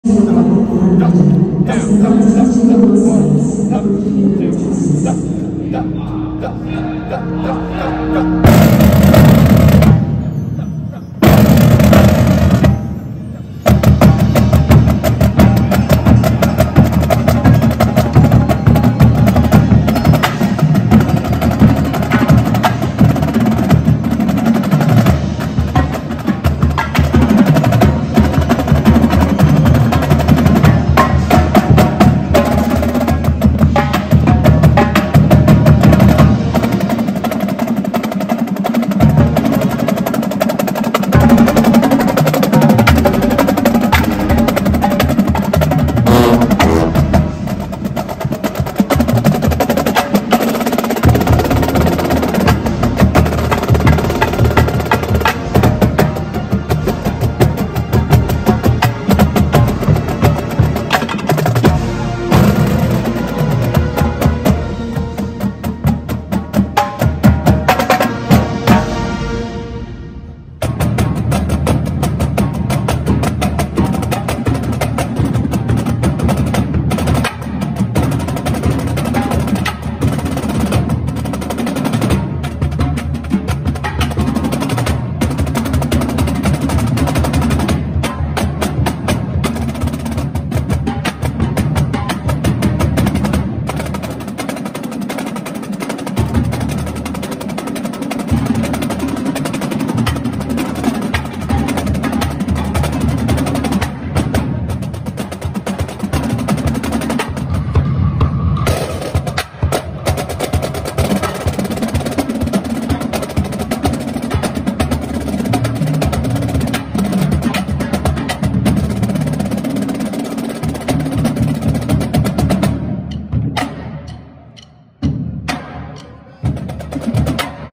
2022 Hendrickson Drumline Grand Nats
2022 BOA Grand National Championship Prelims rehearsal and Finals Warm Up